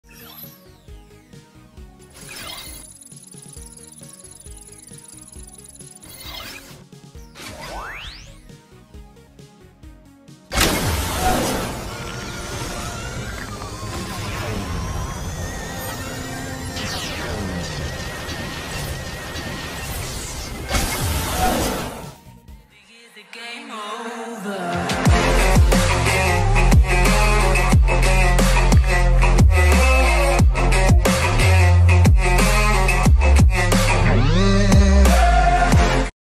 Ben 10 Real Life - Upgrade Transformation!
I had a lot of fun making this Ben 10 Upgrade transformation, he’s one of my more favourable classic Ben 10 aliens.